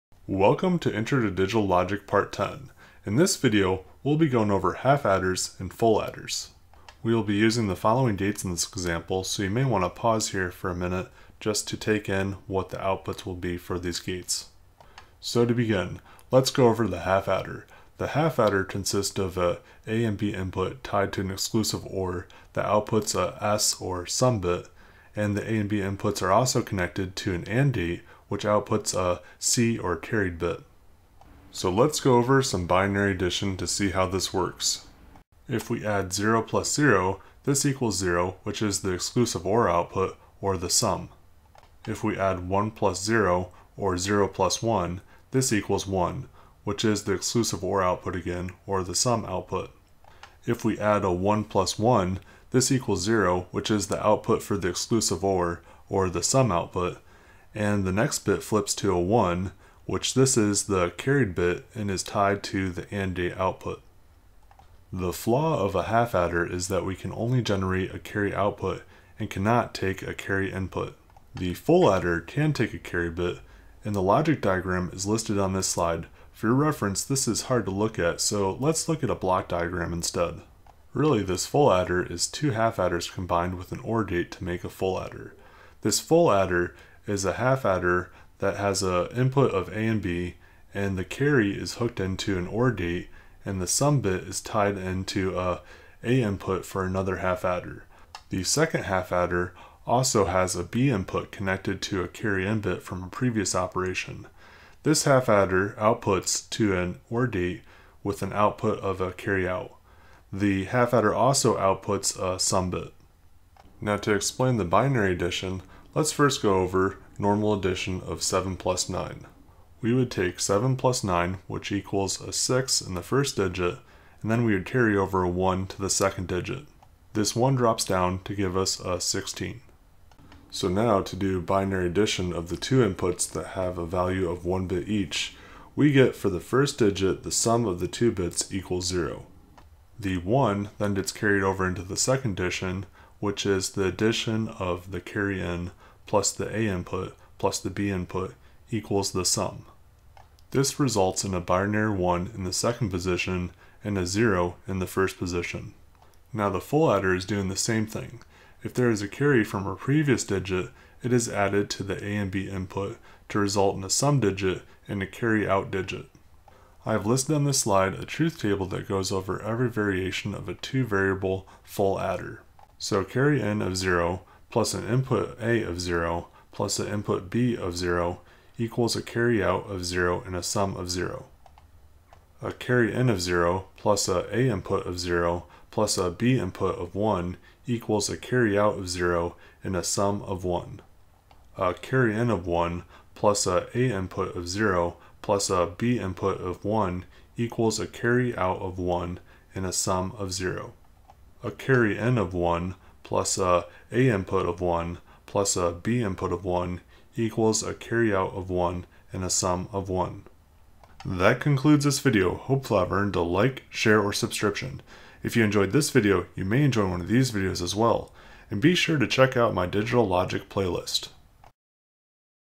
Half Adder and Full Adder Explained (Digital Logic Part 10)
Welcome to intro to digital logic part 10. In this video we will be going over half adders and full adders.
As a refresher here is the logic ouputs for an exclusive or gate
and an gate
and an or gate
So to begin lets go over half adders. Half adders consists of a and b inputs tied  to an exclusive or that outputs a S or sum bit and the a and b inputs also go to a and gate that out puts a carried bit.
So lets go over some binary addition to see how this works.
If we add 0 plus 0 this equals 0 this is the exclusive ore ouput
If we add 1 plus zero or zero plus 1 this equals 1 this is also the exclusive or ouput
If we added one plus 1 this equals zero which is the ouput of the exclusive or and the next bit flips to a 1. The next bit is the carried bit which is and gate carried output.
The flaw of the half adder is that it can only generate a carry ouput and can not take a carry input
The full adder can take a carry bit and the logic gate diagram is listed on this slide for your reference this is hard to look at so lets look at a block diagram.
Really this is 2 half adders combined with an or gate to make a full adder. This is full adder is a half adder with its carry bit hooked into an or gate and the sum bit tied into the a input of another half adder. The second half adder also has b input connected to a carry in bit from a previous operation. This half adder outputs to an or gate with an output of carry out and the  an ouput of a sum bit.
Now to explain the binary addition lets first go over normal addition of 7 plus 9.
We would take the 7 plus 9 and get a 6 then we would carry over a 1
This one drops down to give us 16
So now do a binary addition of 2 inputs that values are both 1 bit we get for the first digit the sum of these 2 bits equals zero
The 1 then gets carried over to the second digit addition which carry in plus a plus b equals the sum
This result in binary 1 in second position and 0 in the first position.
Now the full adder is doing the same thing if there is a carry for a previous digit it is added to the a and b input to result  in a sum digit and a carry out digit
So an carry in of 0 plus a a in put of 0 plus a b input of zero equals a carry out of zero and a sum of zero
a carry in of 0 plus a a in put of 0 plus a b input of 1 equals a carry out of zero and a sum of 1
a carry in of 1 plus an a in put of 0 plus a b input of 1 equals a carry out of 1 and a sum of 0
a carry in of 1 plus an a in put of 1 plus a b input of 1 equals a carry out of 1 and a sum of 1